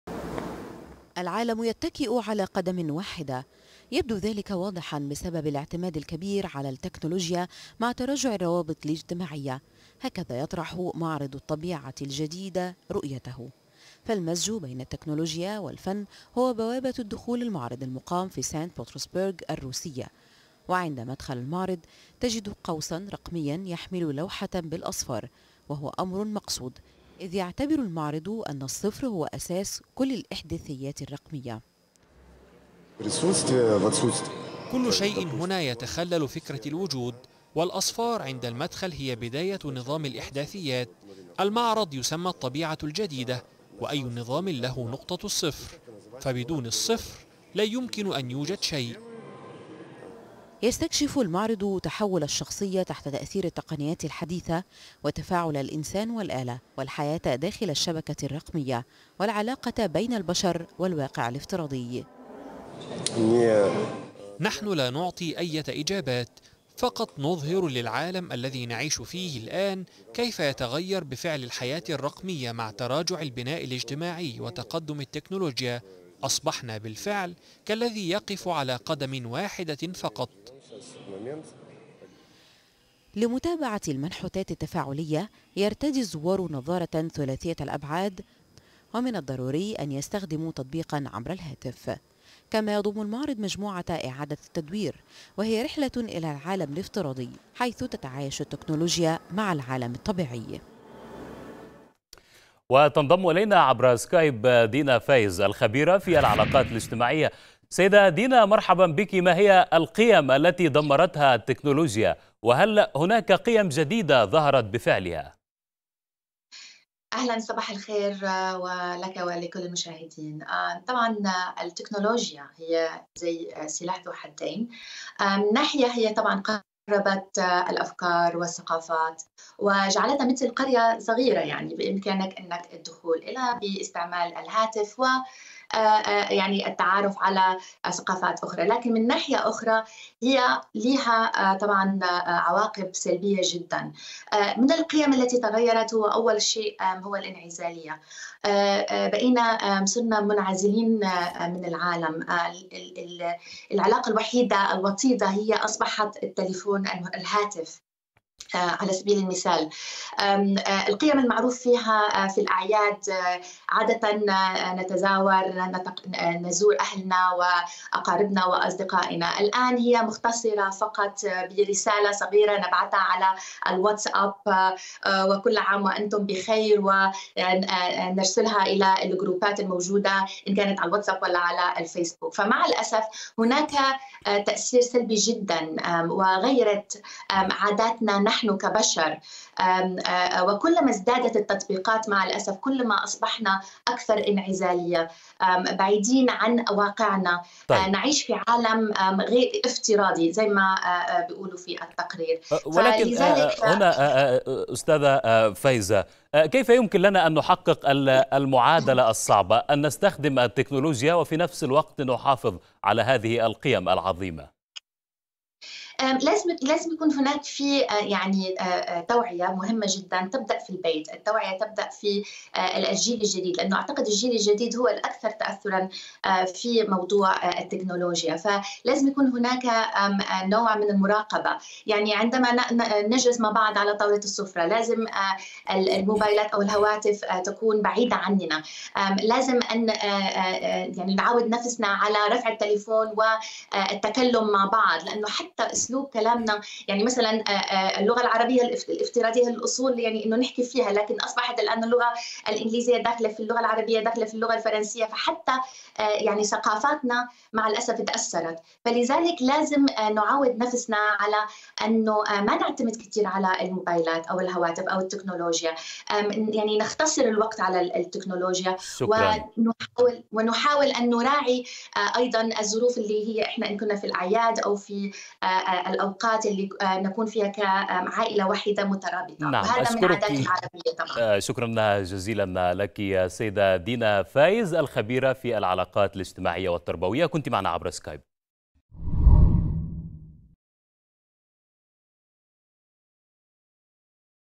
معرض روسي يؤكد أن التكنولوجيا تضع الروابط الاجتماعية في خطر.. هل من حلول؟
يوما بعد يوم، يتأكد لنا أن مستقبل العلاقات الاجتماعية في خطر، نتيجة اقتحام التكنولوجيا الرقمية كل تفاصيل حياتنا اليومية. تحذير ألقى عليه الضوء معرض في مدينة سانت بطرسبرج الروسية، والذي يحاول استكشاف تأثيرات التفاعل بين البشر والتكنولوجيا.
المزيد من التفاصيل يتحدث عبر السكايب  دينا فايز الخبيرة في العلاقات الاجتماعية والتربوية، عبر النشرة الإخبارية